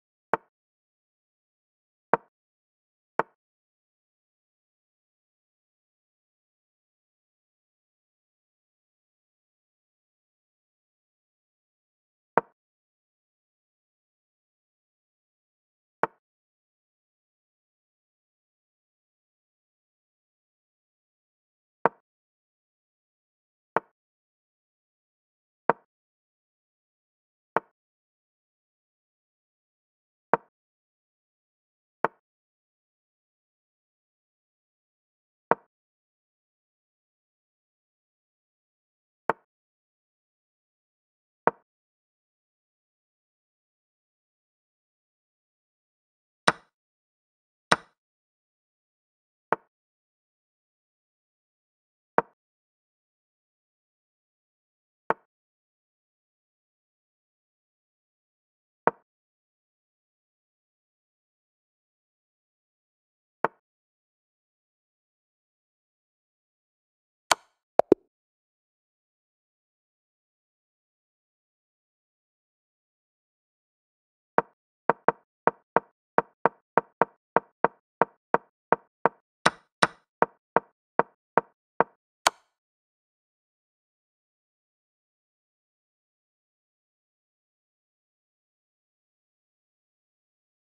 Joey de jesus Chess gameplay KINGS PAWN OPENING 3 minutes Blitz CHECKMATE in 12 moves
to support my GM journey :)